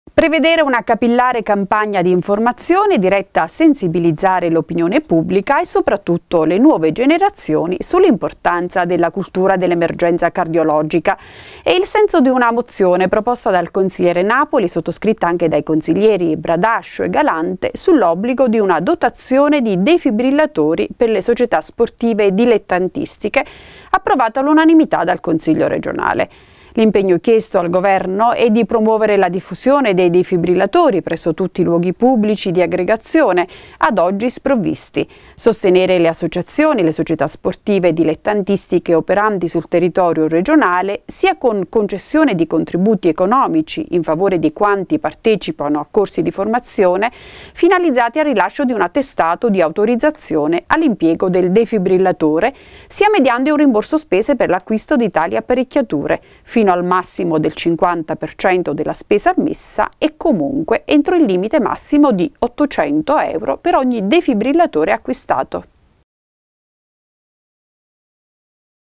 DEFIBRILLATORI IN LUOGHI PUBBLICI, SI' CONSIGLIO A MOZIONE
Diffondere la cultura dell'emergenza cardiologica